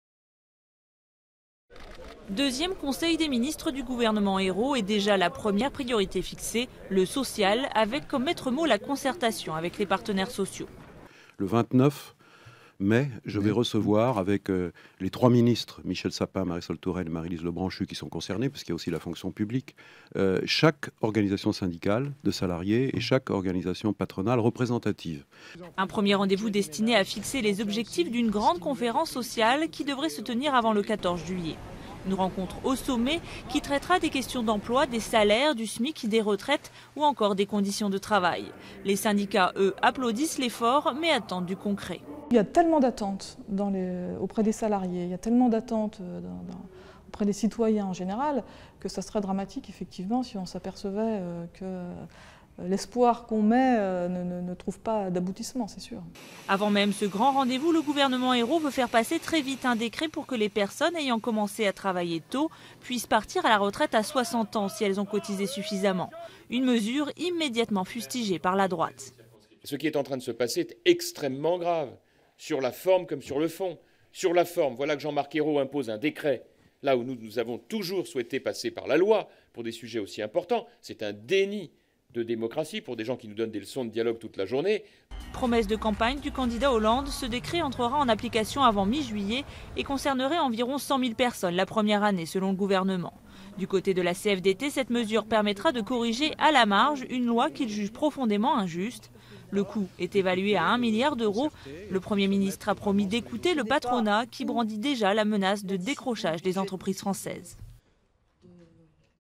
Le social: priorité du gouvernement Ayrault
Le Premier ministre, Jean-Marc Ayrault, a mis mercredi le social en haut de son agenda: une semaine après sa nomination, il a fixé la méthode de concertation avec les partenaires sociaux et ouvert le premier chantier de réforme des retraites, suscitant une violente réaction de la droite qui parle d'un "déni de démocratie".  Durée: 01:49.